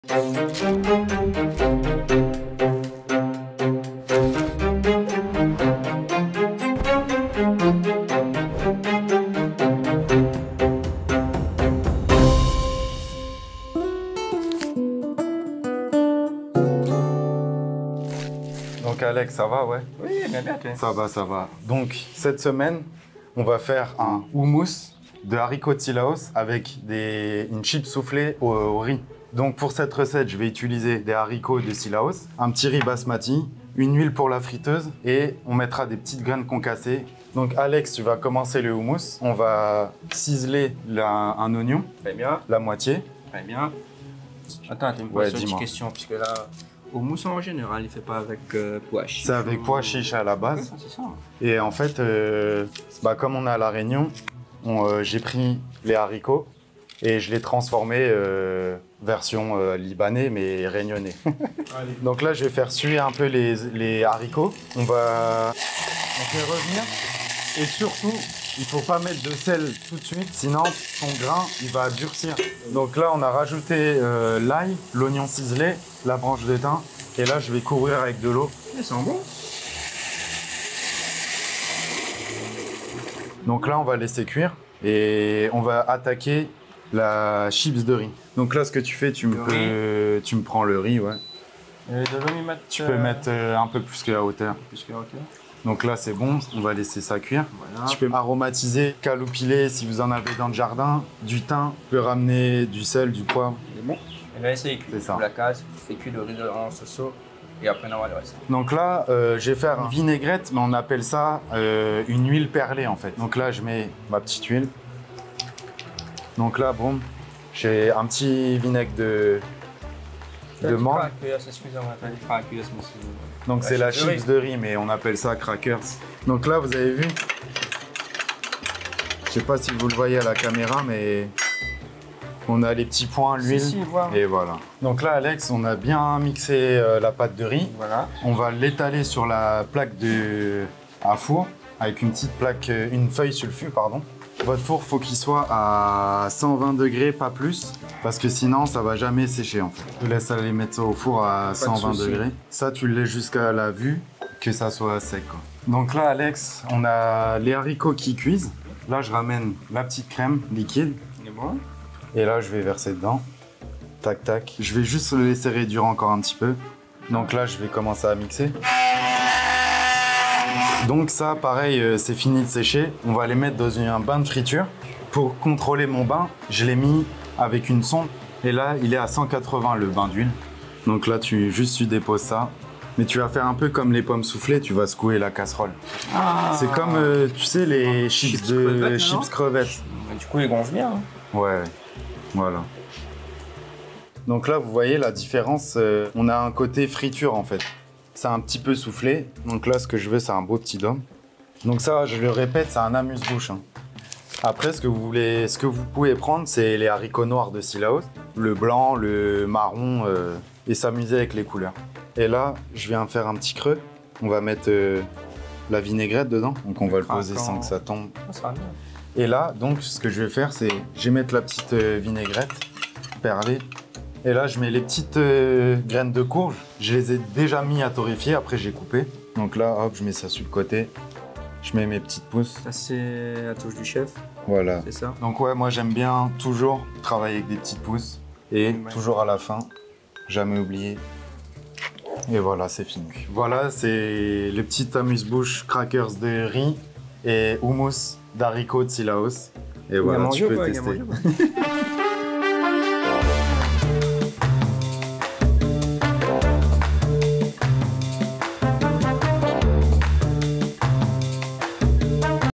Amuse Bouche HOUMOUS HARICOTS DE CILAOS ET SON CRAKER'S DE RIZ
Recette autour d’un amuse bouche avec Julien et Alexandre